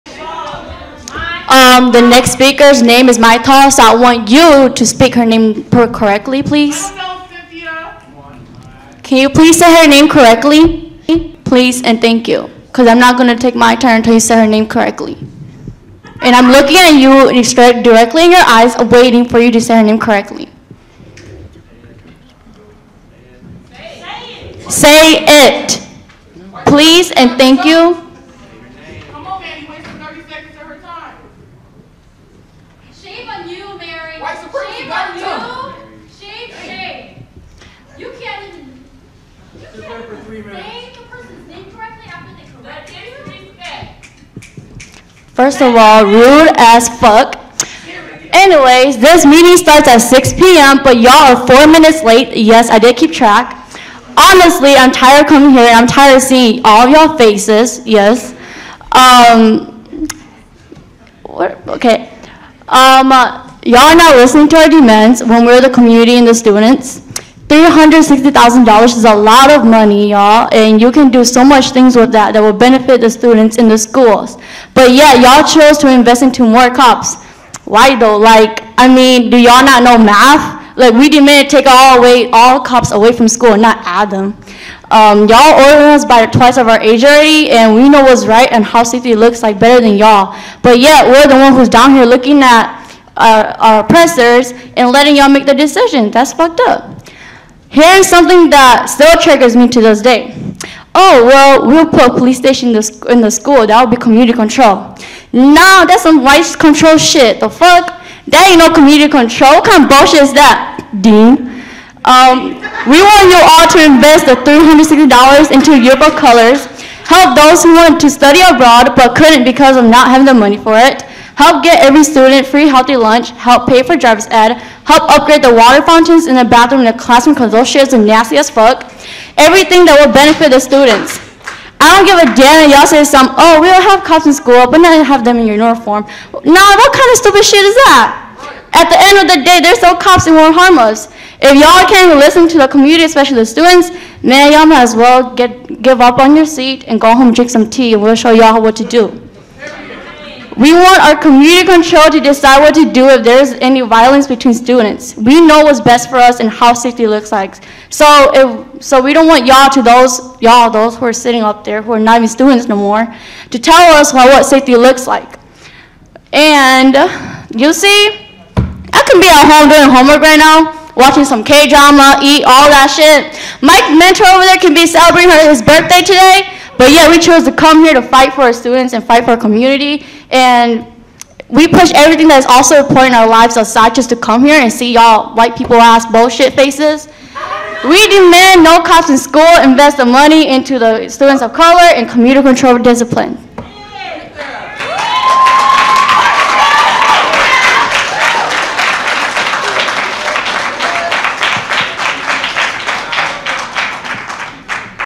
SMUG SJW Sneers At School Board "I'm tired of seein all y'all faces!"
You don't listen to, or cave to our rudely, and aggressively delivered "demands"  !?!?!?!?!?  WHUTTTT!!!???